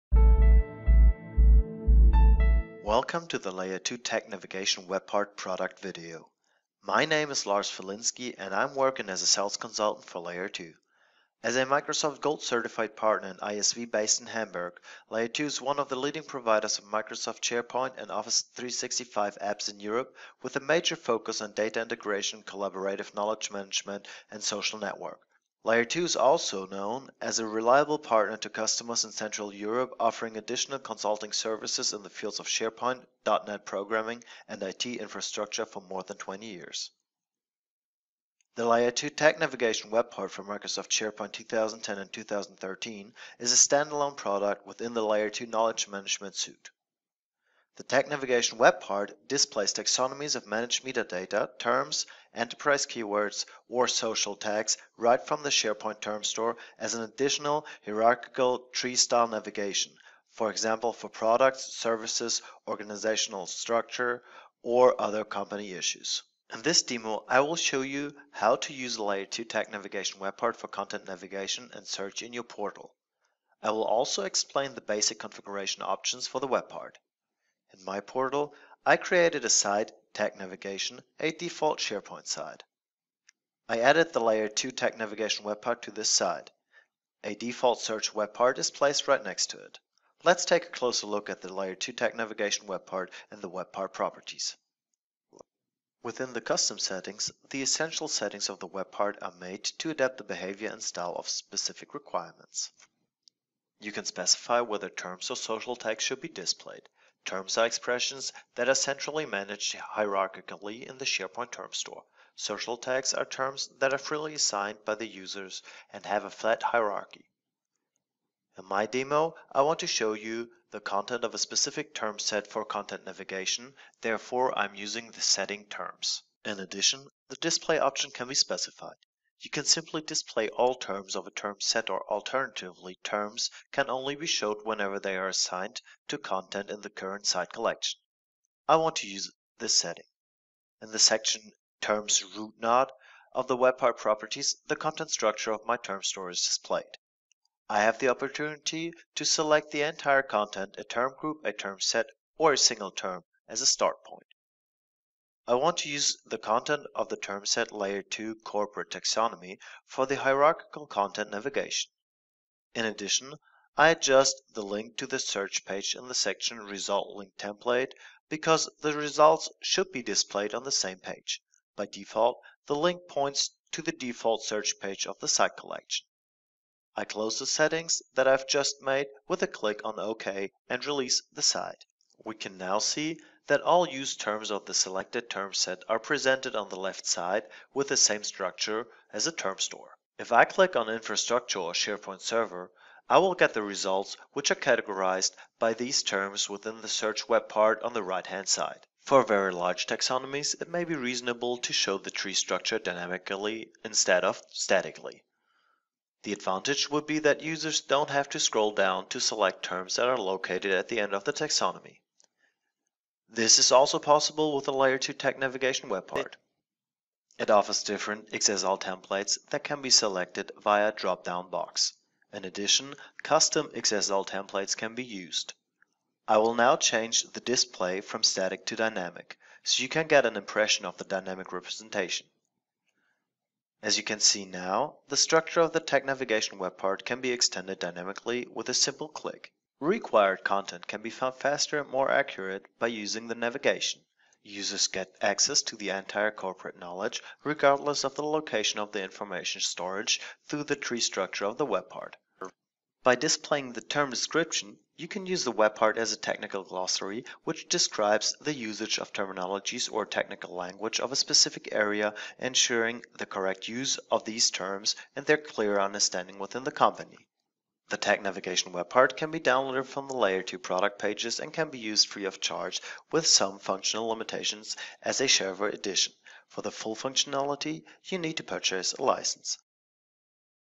SharePoint Term Set Navigation: Layer2 Tag Navigation Web Part
Features:

• More efficient information access on SharePoint portals.
• Simply use tree-based SharePoint Term Sets for content navigation
• Flexible setup and design via XSL.
• Flexible linking to profile or wiki pages, or search.
• Helps to navigate libraries that have more than 5.000 items (list view threshold).
• External Data can be included in index.

The Layer2 Tag Navigation Web Part for SharePoint displays taxonomies of managed metadata, terms, enterprise keywords or social tags right from the SharePoint term store as additional hierarchical, tree-style navigation. The Web Part offers an additional content navigation to browse the corporate knowledge hierarchically - cross site-collection and independent of the content store's structure. Terms or tags can be linked to tag profile pages, hashtag profile pages, Wiki pages, any custom pages or directly to the tagged content via search. The output can be customized using flexible XSLT-based templates, static and dynamic (for larger taxonomies). The Web Part goes far beyond the out-of-the-box metadata navigation features of Microsoft SharePoint Server 2010 and 2013, where pages can be assigned to each metadata item manually only. Please note, that a flat alphabetical (A-Z) Tag Directory Web Part is available as well.